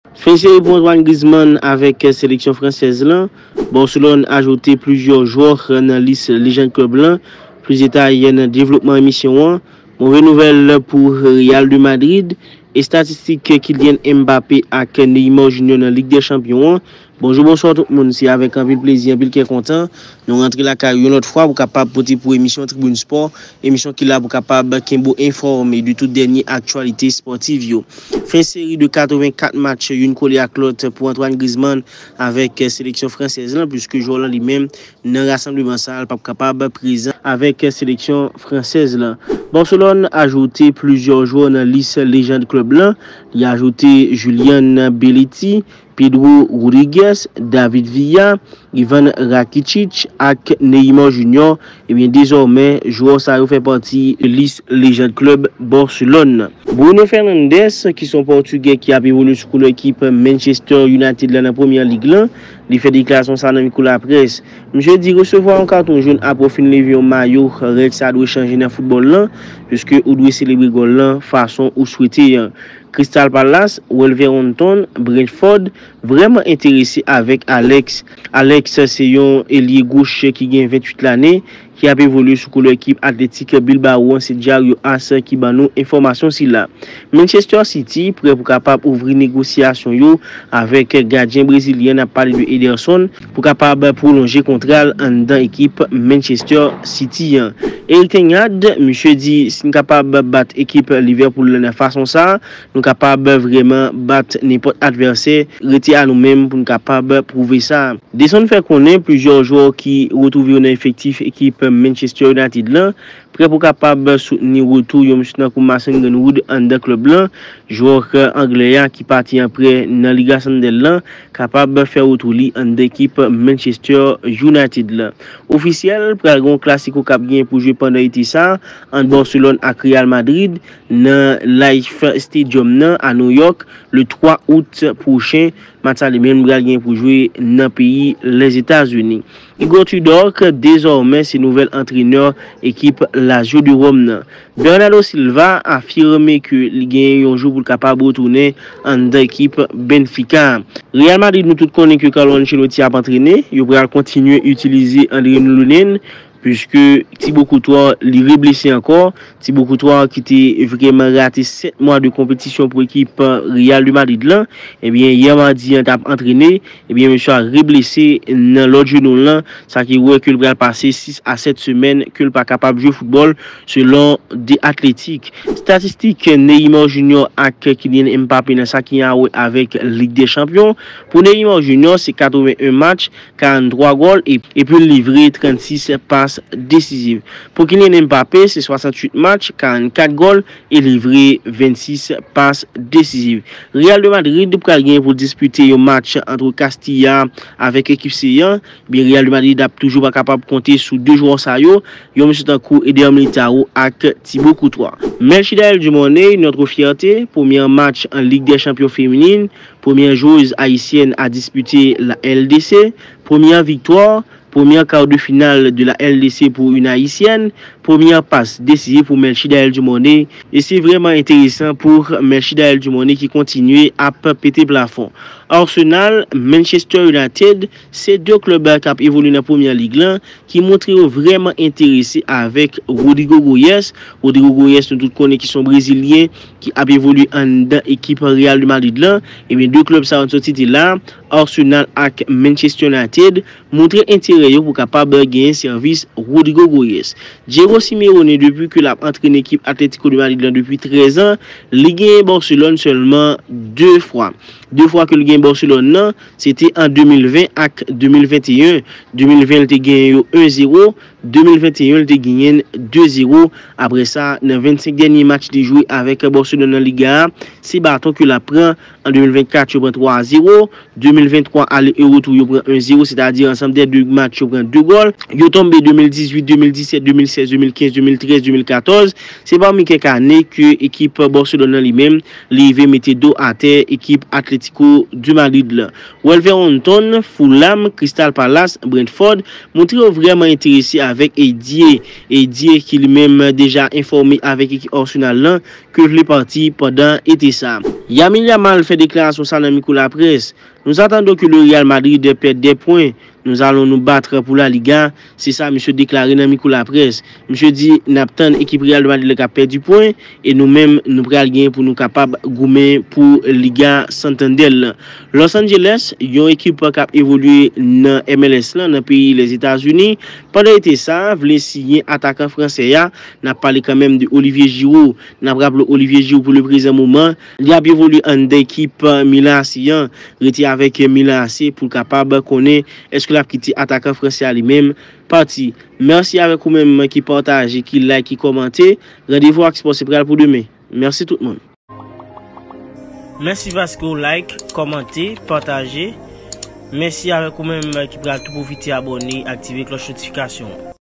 Move nouvèl pou ekip Real madrid la /Neymar estatistik entèresan pou jwè brezilyen an
🟢Fen seri pou Antoine Grizman n avèk seleksyon fransèz la .

🟣Barcelone ajoute plizyè jwè nan lis lejand klèb la plis detay nan devlopman emisyon an .

🟡Move nouvèl pou ekip Real madrid .

🔵Estatistik Mbappé ak Neymar nan lig dè chanpyon an .